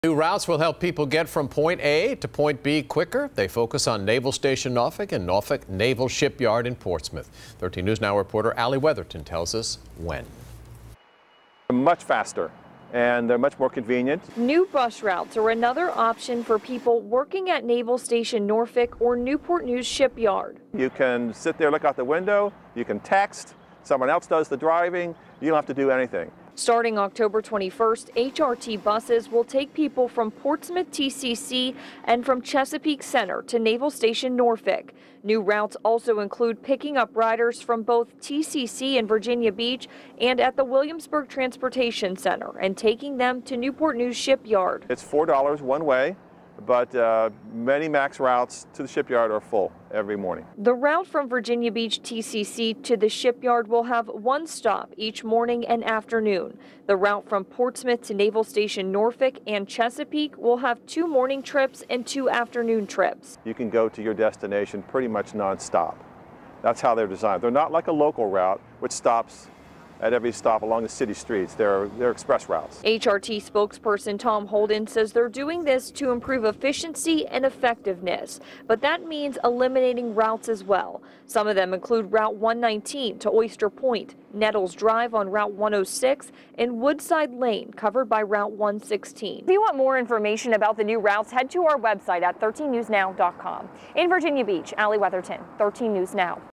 Changes coming to HRT routes, schedules
HRT rolls out brand new bus lines, promising to streamline transit service in Hampton Roads